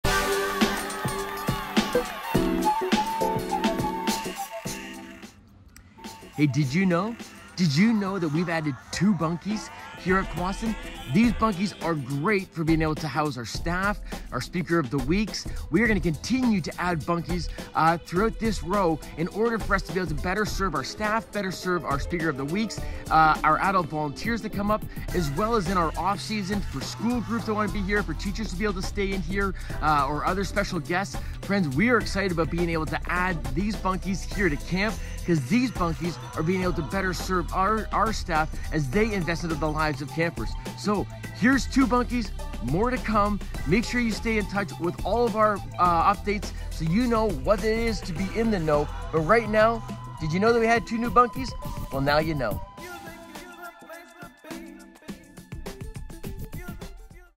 DID You Know Part 7 - New Bunkies at Kwasind
We've added new BUNKIES at Kwasind to better serve our staff, volunteers and speaker of the week as we grow into the future